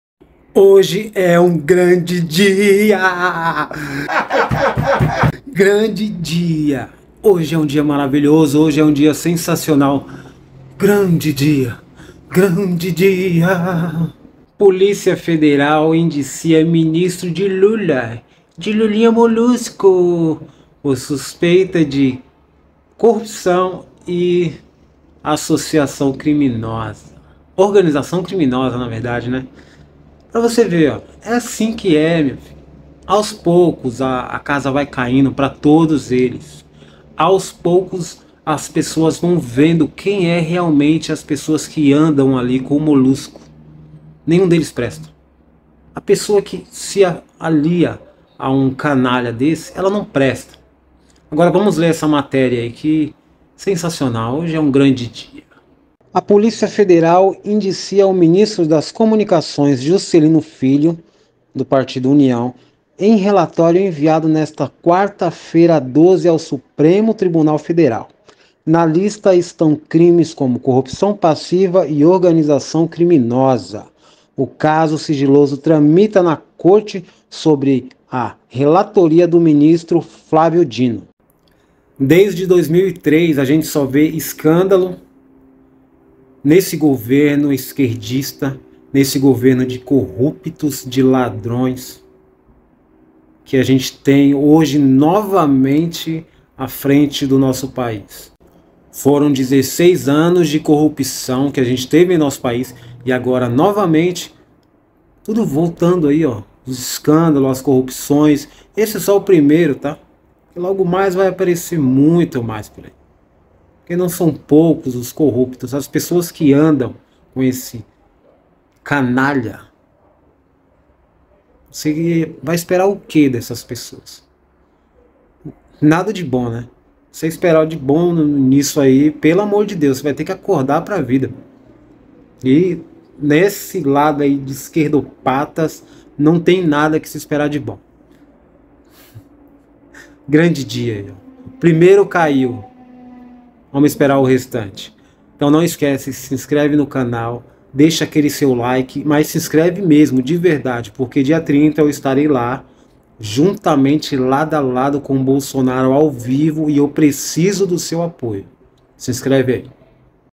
POLÍCIA FEDERAL Indicia MINISTRO de LULA - COMEÇOU
Polícia Federal indicia ministro de Lula. Começou a cair os ministros de Lula, será que a polícia federal vai achar mais coisas?